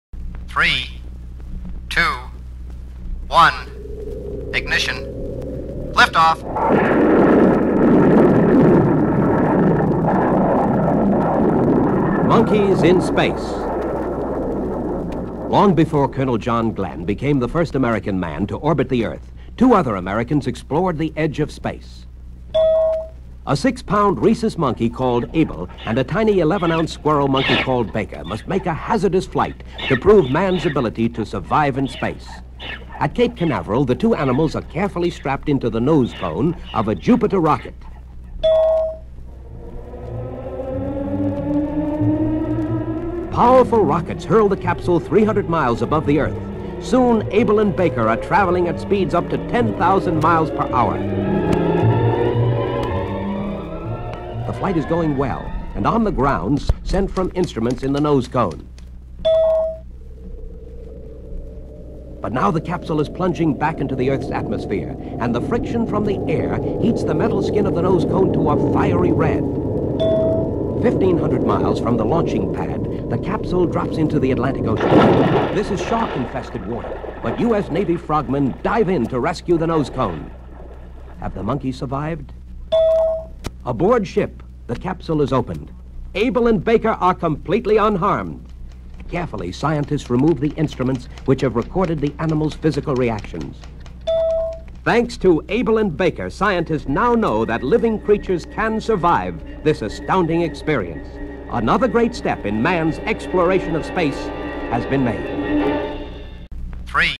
01 - Monkeys In Space
Slide one of Kenner's Give-A-Show Adventure Time set from 1962, "Monkeys in Space".  See more about Give-A-Show at the Give-A-Show Blog